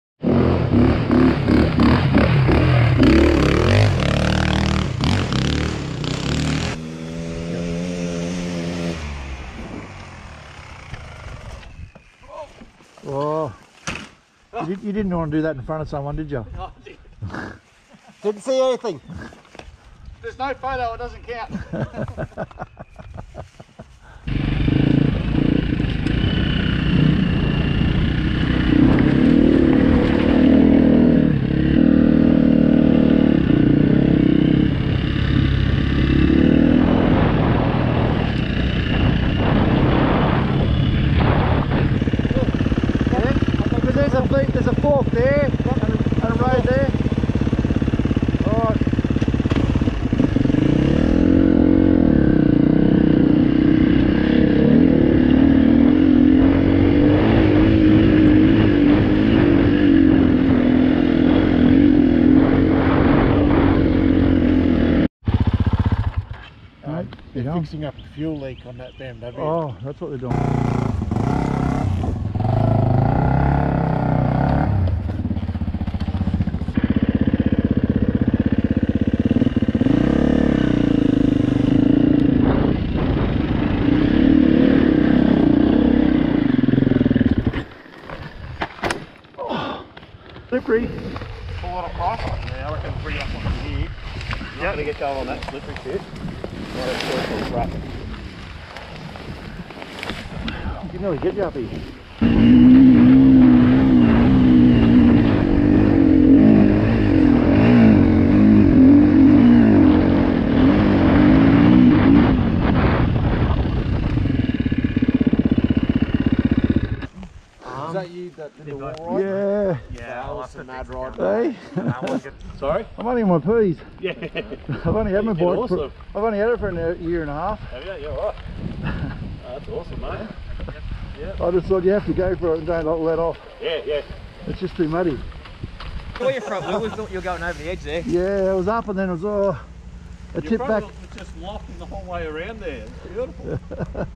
Open Road Rally Lithgow 2023
First rally!
Day Two: Lots of mud, dirt and spills. and a lot of fun!